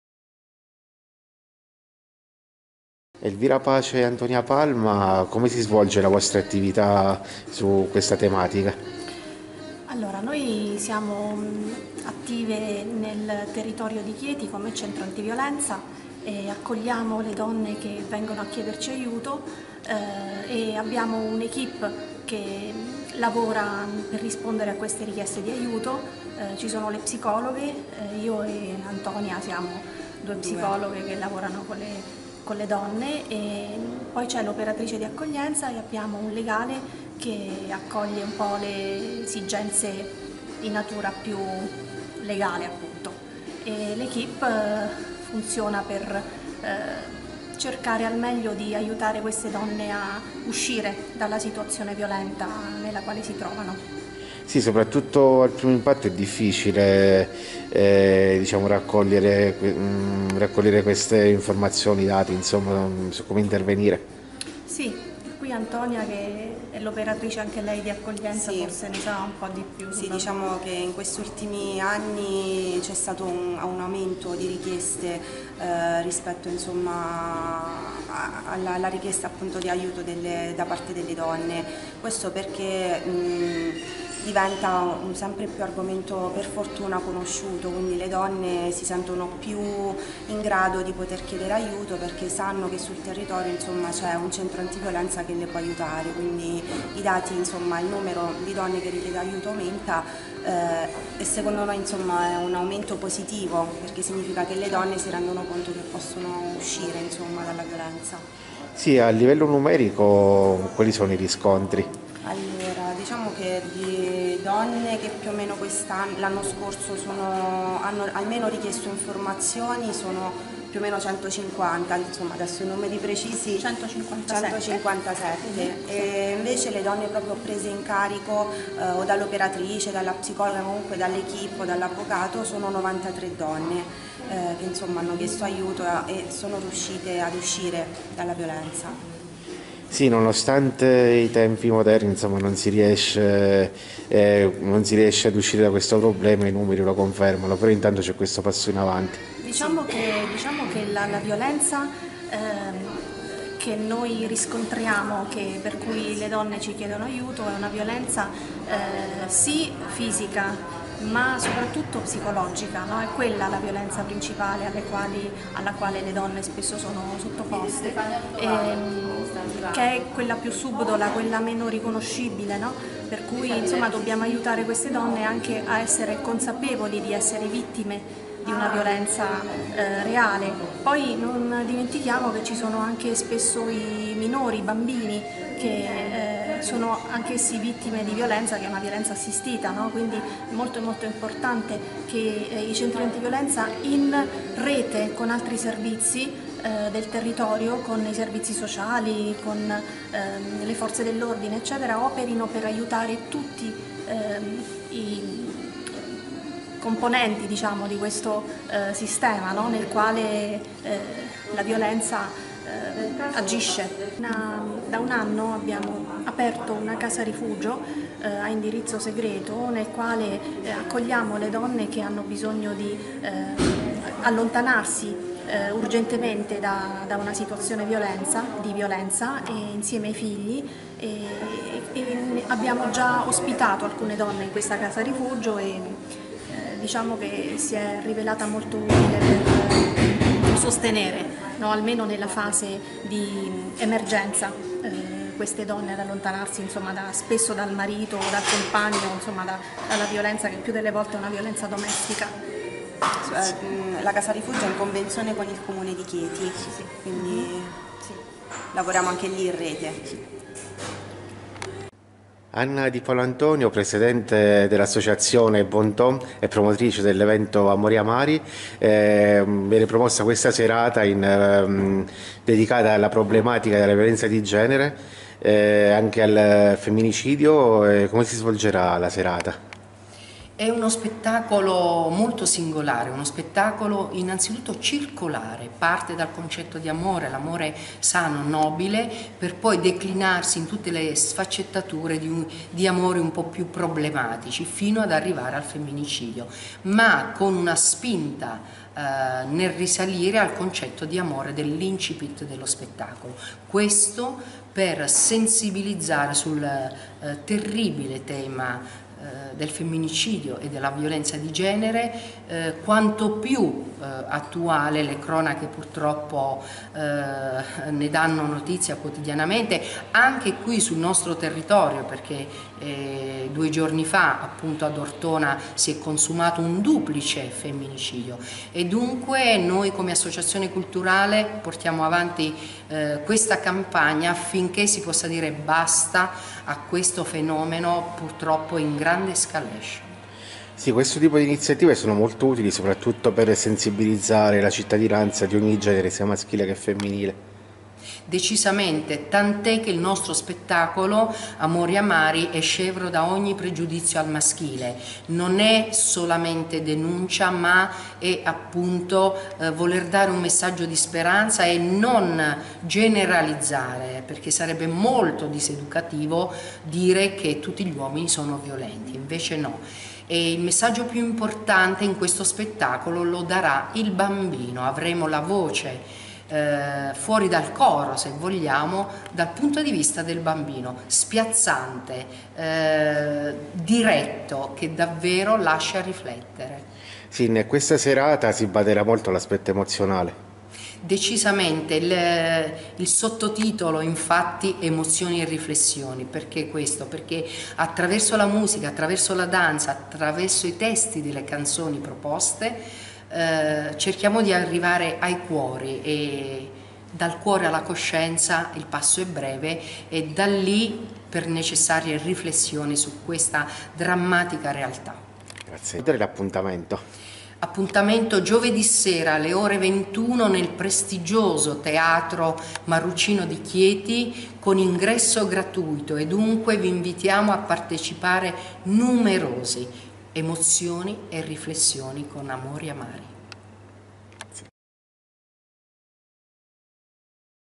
Presentazione spettacolo Amori amari
Le interviste alle psicologhe del Centro Antiviolenza Alpha, Elvira Pace e Antonia Palma. e alla presidente dell'associazione Bon Ton, Anna Di Paolantonio